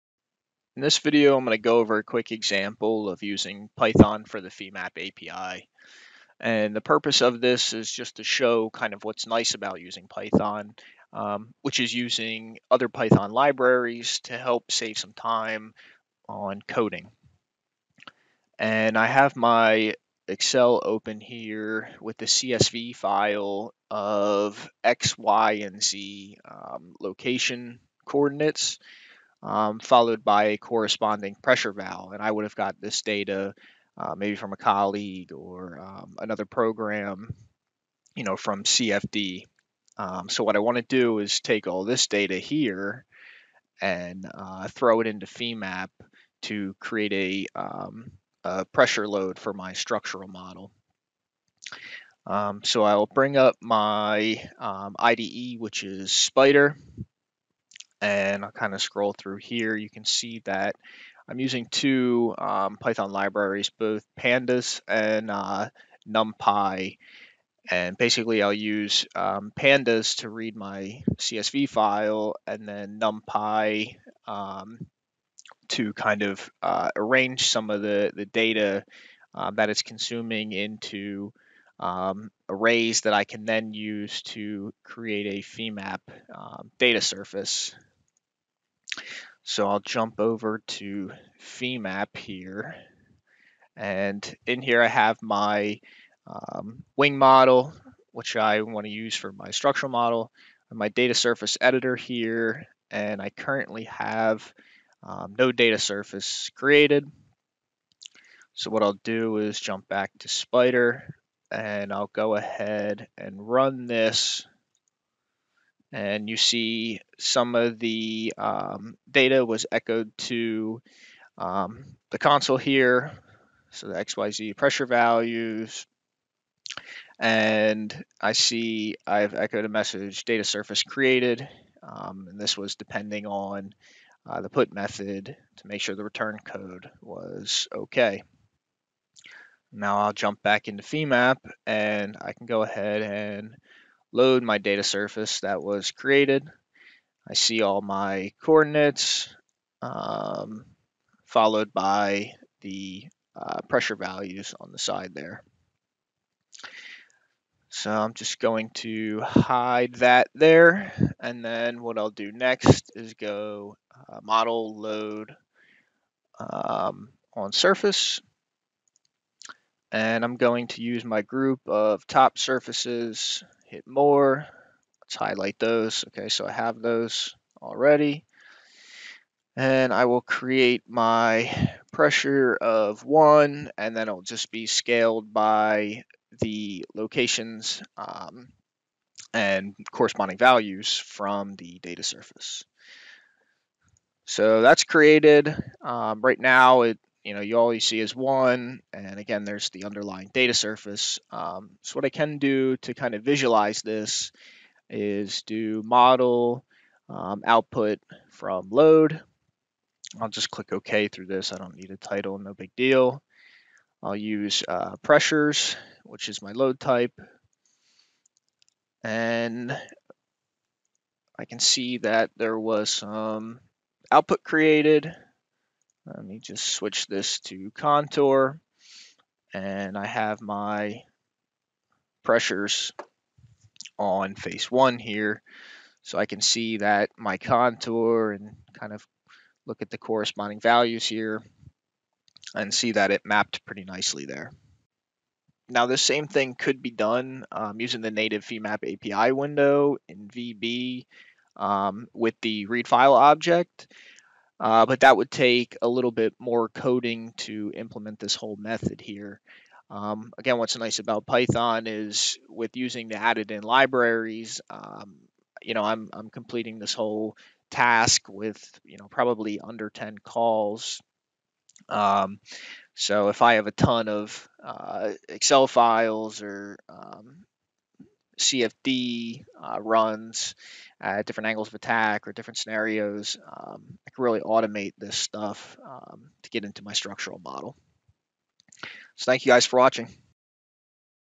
Femap API using Python
A short example going over why you might want to use Python for the Femap API.